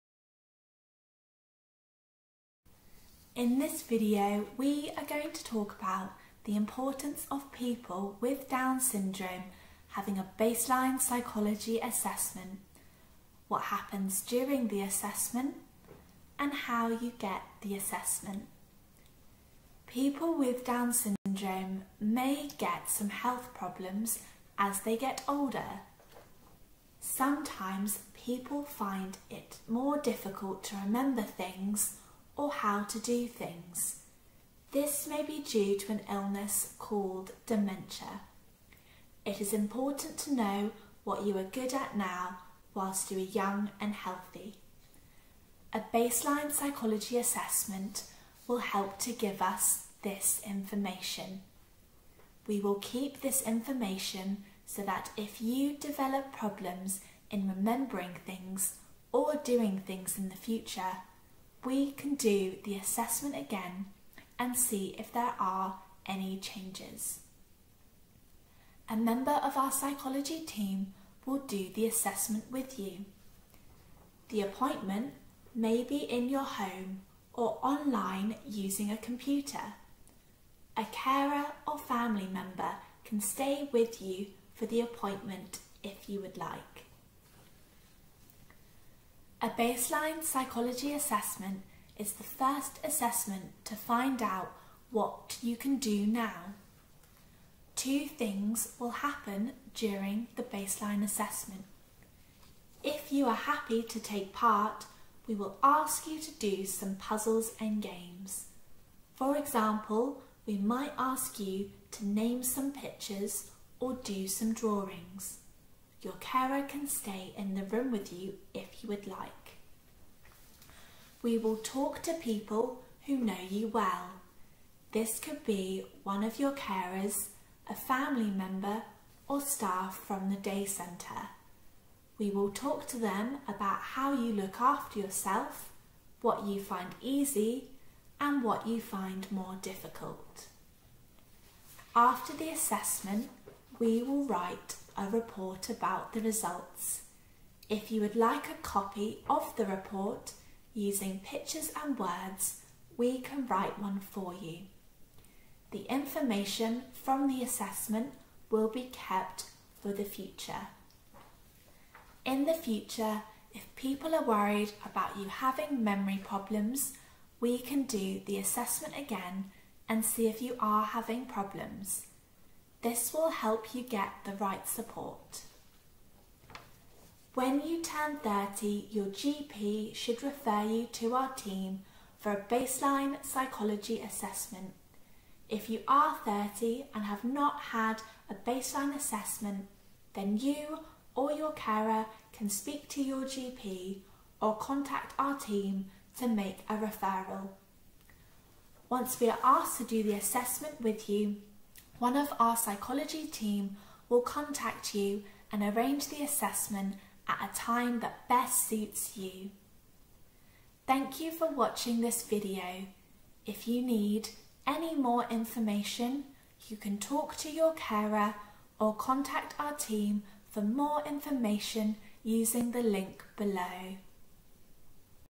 Explaining a baseline psychology assessment for people with Down’s syndrome
This video explains why we want to offer people with Down’s syndrome a baseline psychology assessment, and what happens during an assessment.